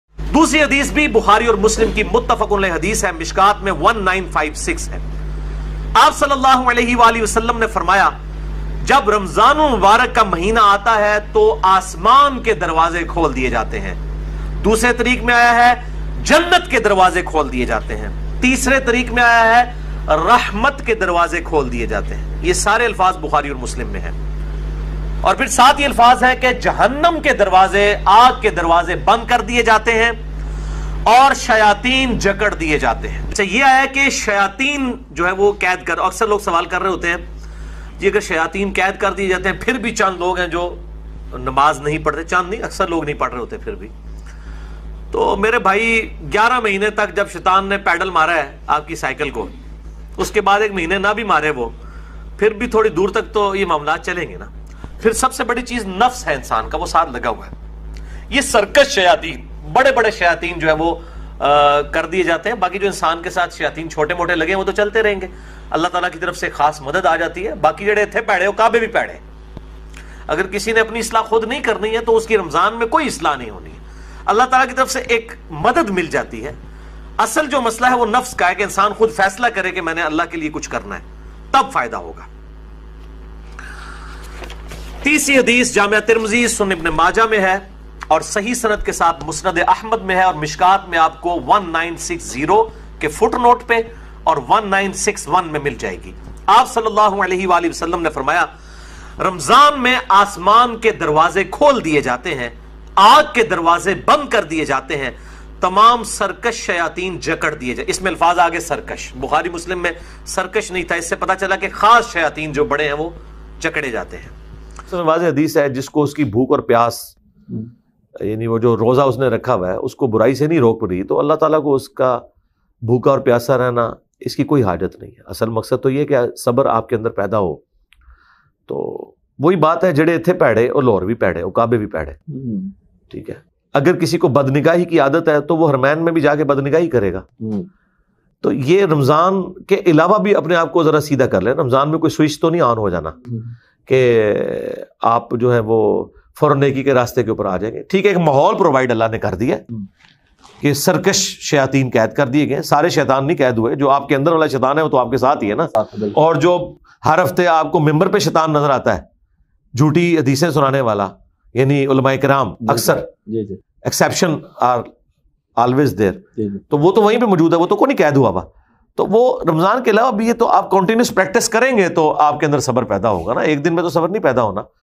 If the DEVILS are IMPRISONED in RAMADAN , then why do PEOPLE commit SINS ??? Eng Muhammad Ali Mirza
Ramzan ki fazilat by Engineer Muhammad Ali Mirza